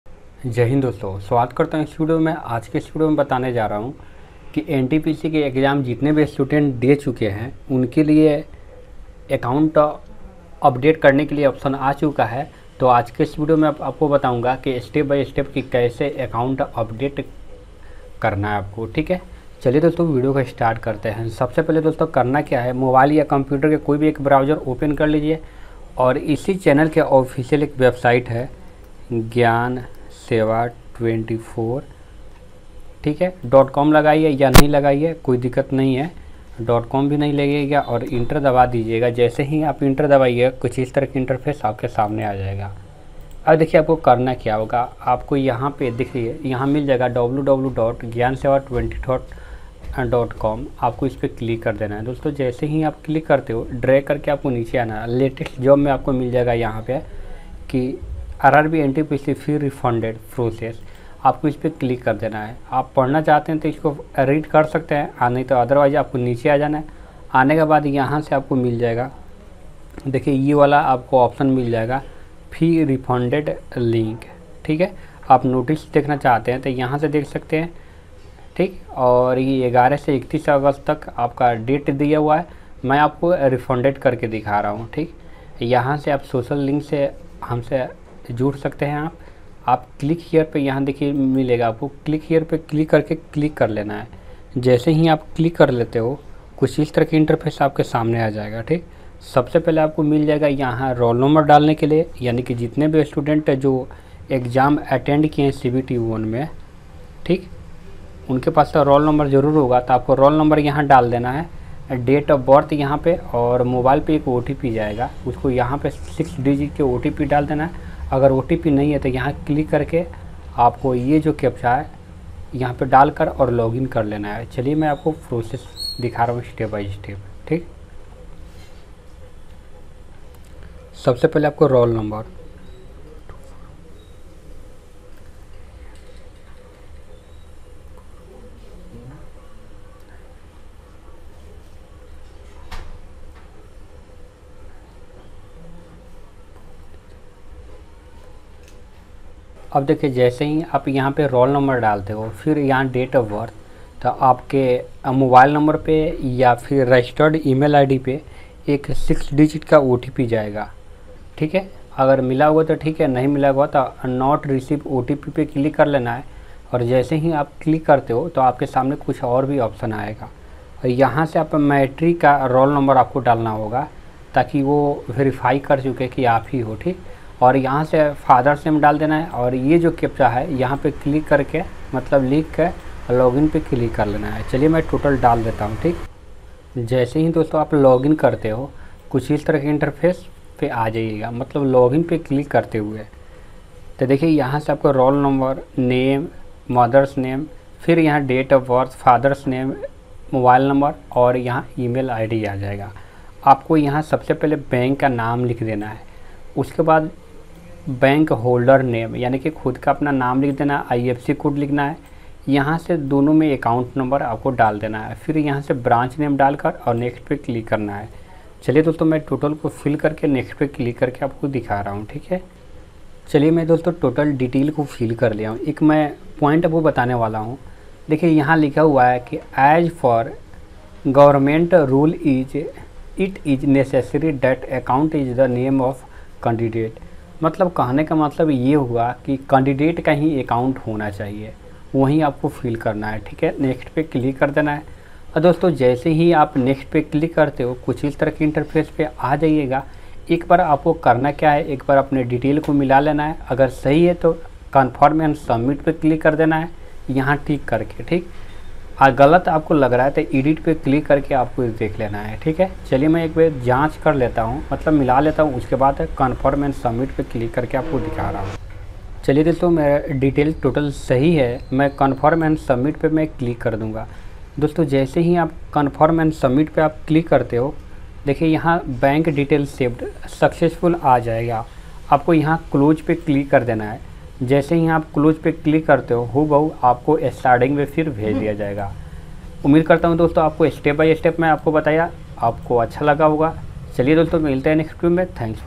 RRB NTPC Fee Refounded Process फीस रिफंड का ऑफिशियल नोटिफिकेशन जरी ऐसे करे ऑनलाइन
Latest Govt. Jobs Updates/Form, Admit crard, Results, School or college Updates,
Daily Video uploads times
Morning 12:00am